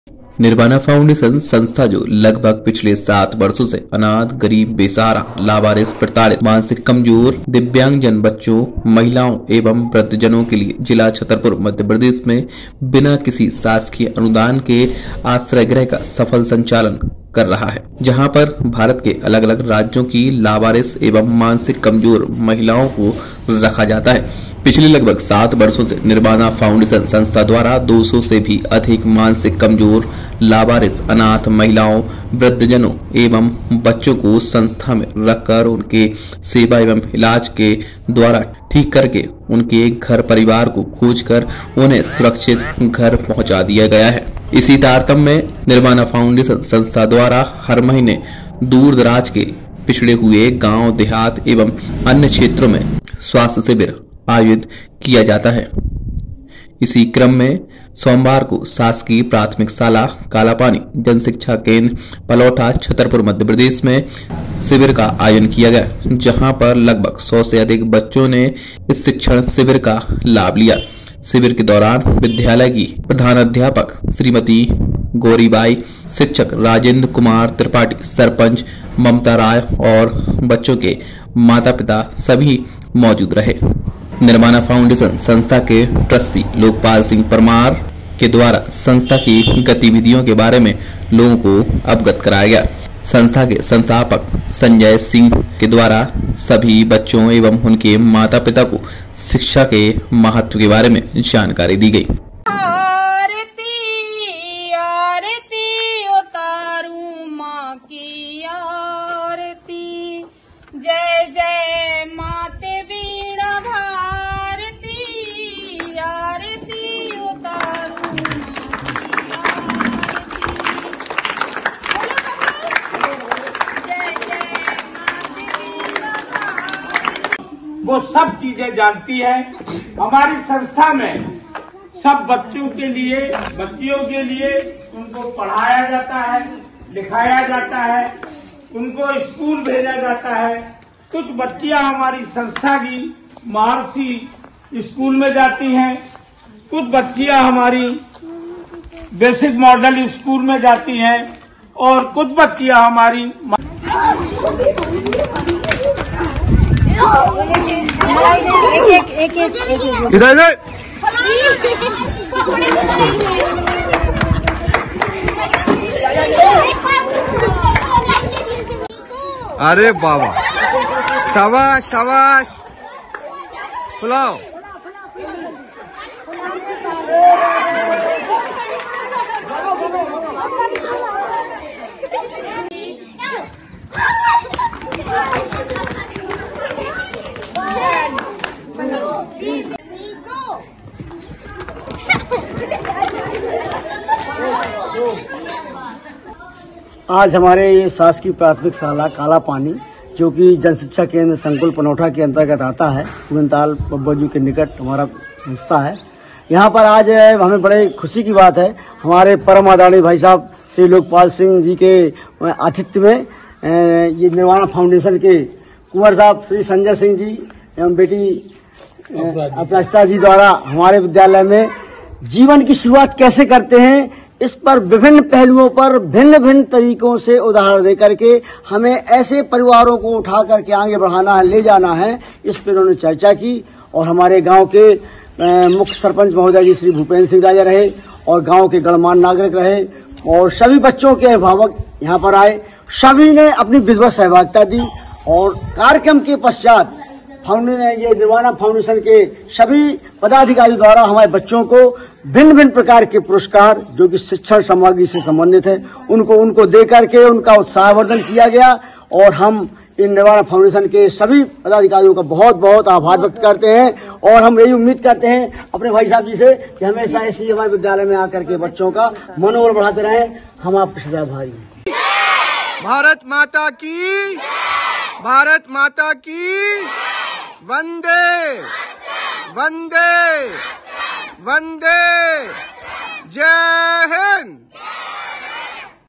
छतरपुर - निर्वाना फाउंडेशन ने लगाया शिक्षण शिविर बच्चों और महिलाओं को बांटी सामग्री।
छतरपुर - निर्वाना फाउंडेशन ने लगाया शिक्षण शिविर बच्चों और महिलाओं को बांटी सामग्री।citynews18 city_news18 citynewschhatarpur chhatarpur chhatarpurnews citynews18chhatarpur news18 #18